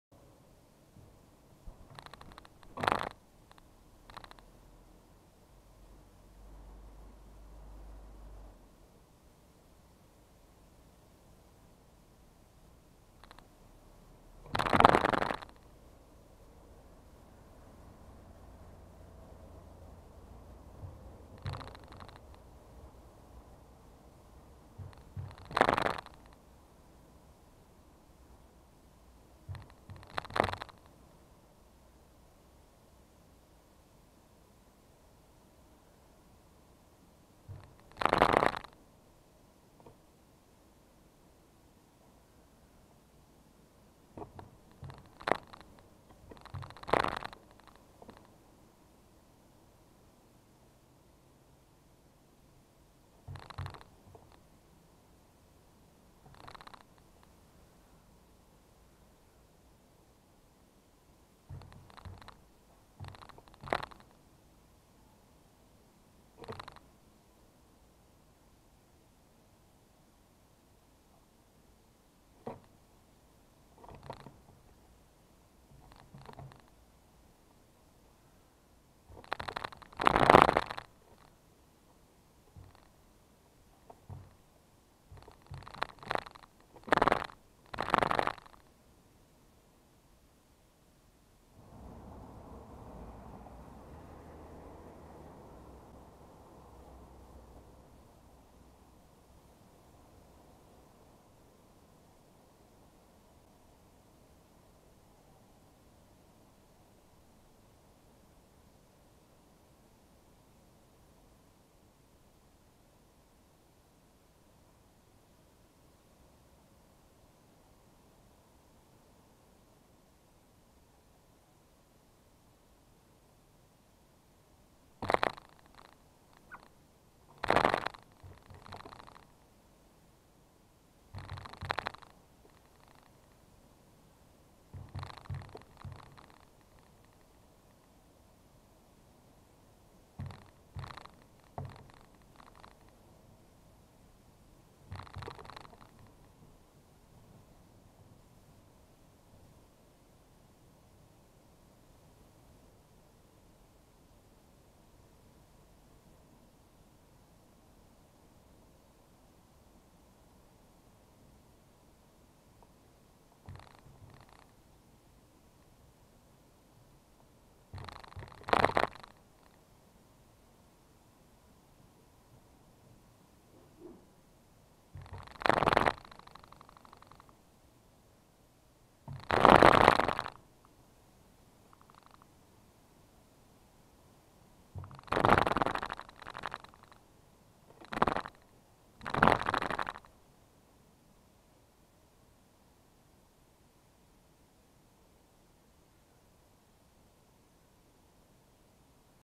Jay in the Snow - Protein Breakfast @ Feeder
Early morning chowing down on a bit of smoked pork. Birdies need their protein in the winter...